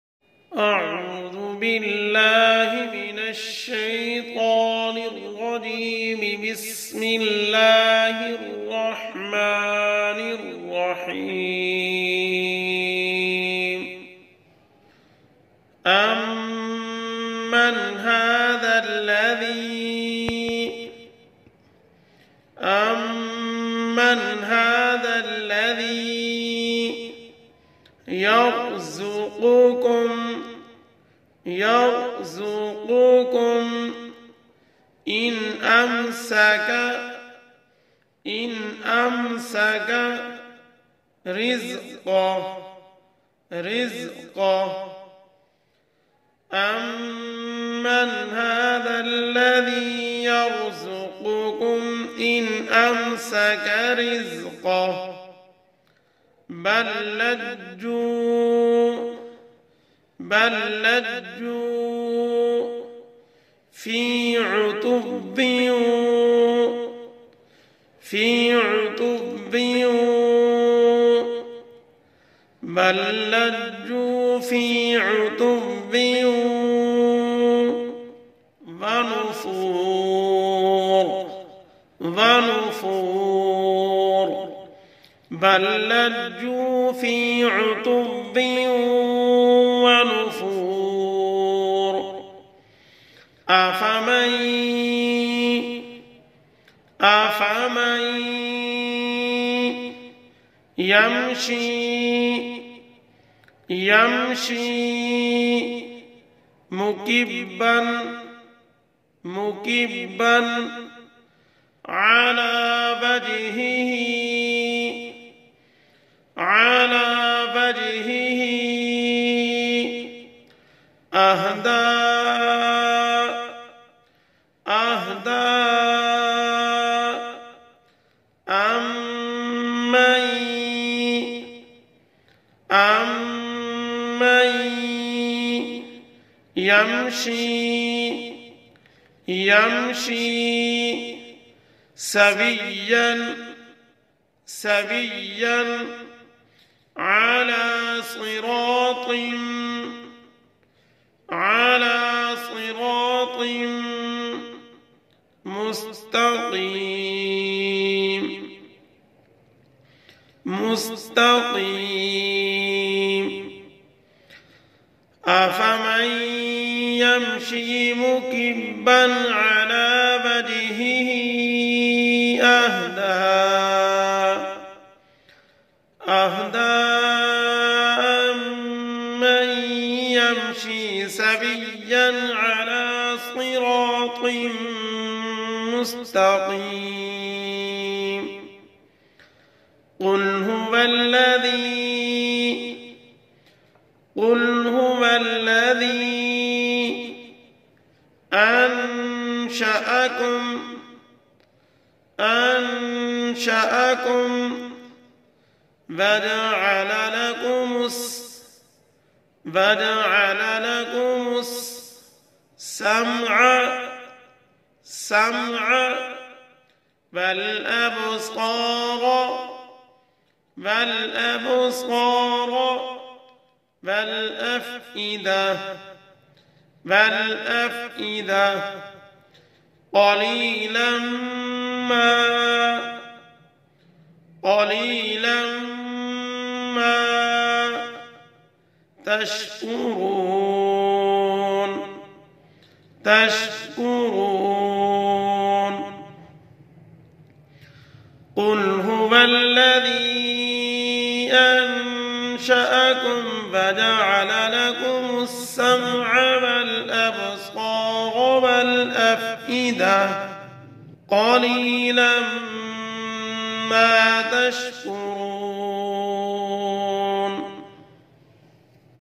Read Surah Al Mulk Part 7 | Surah Mulk Word by Word | سورۃ الملک لفظ بہ لفظ سیکھے
Read Surah Al Mulk Part 7 | Surah Mulk Word by Word | سورۃ الملک لفظ بہ لفظ سیکھے | القرآن کلاس
Al Quran class Tilawat Quran
learn Quran class